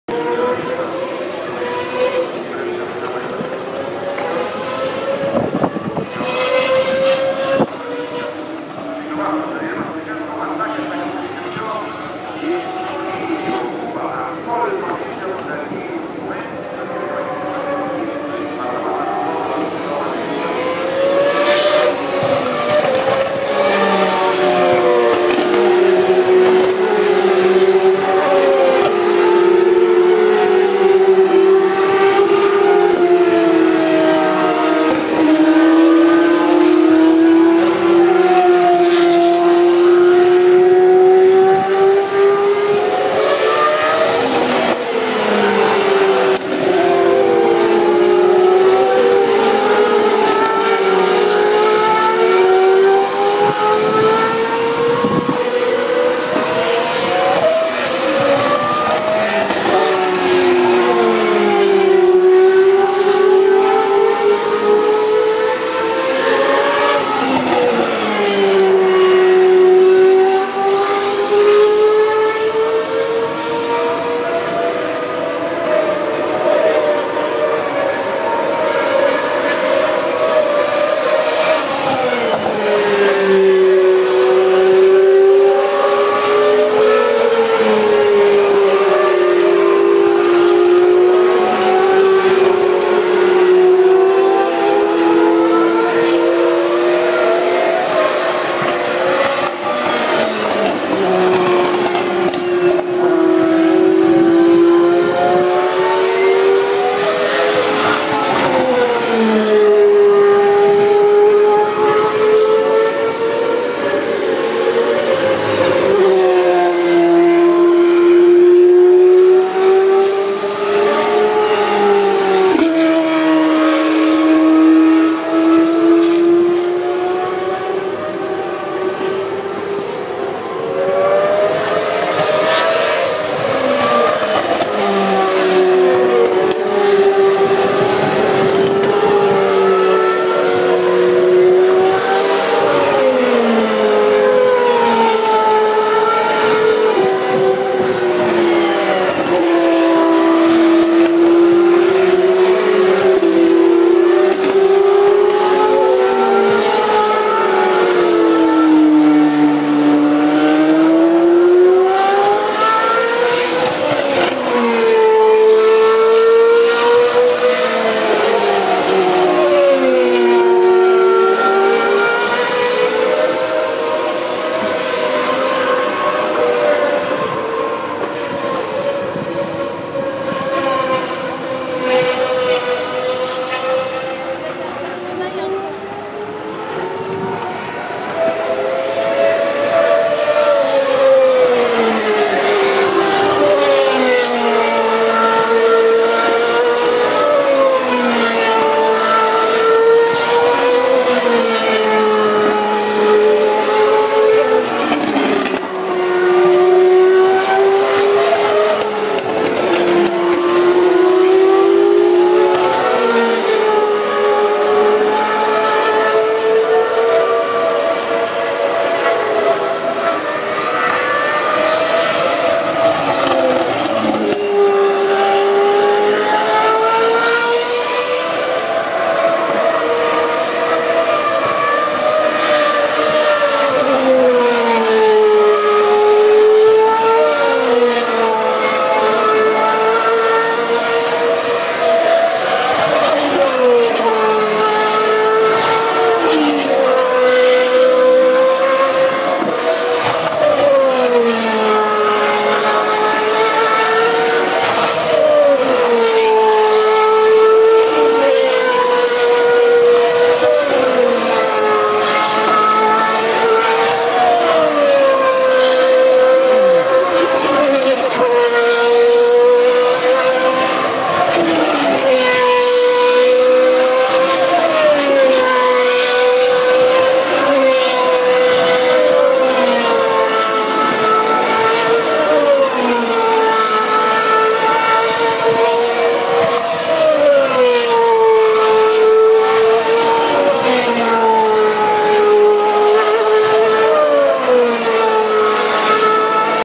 F1 2014 España inicio clasificacion GP2 Series / F1 Spain GP2 qualifying
Este es un video del inicio de la clasificacion de la GP2 en donde Richelmi fue el mas rápido con un 1:29:200. / This is a video of the start of the qualifying of GP2 Series